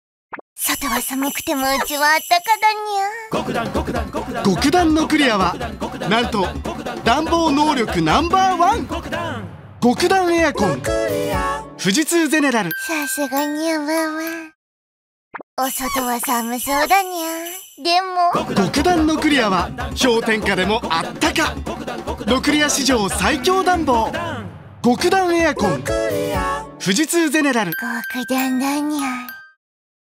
声優・上坂すみれ、暖房でとろけそうなネコの声を演じるWEBCMが登場
声優の上坂すみれをキャラクターボイスに起用した暖房強化型エアコン「ゴク暖 ノクリア」のWEBCM（2種）が、16日から配信。